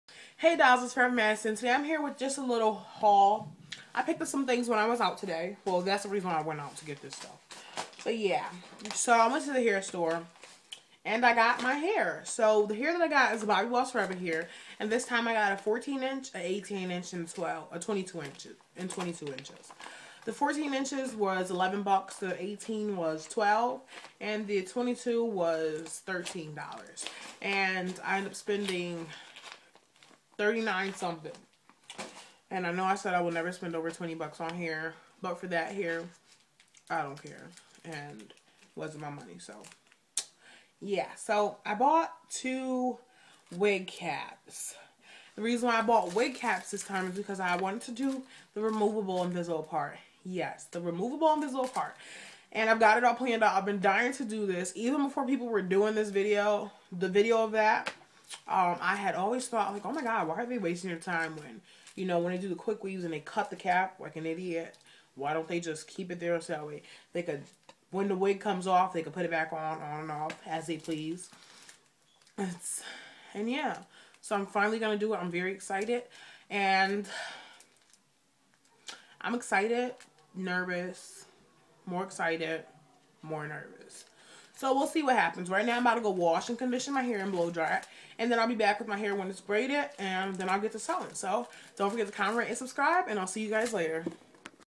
BBS Haul/ sneak peak at new hairstyle
3 packs of Bobbi boss forever 14,18,22 color #2
Black wig cap

Everything in this video was purchased with my own money ;p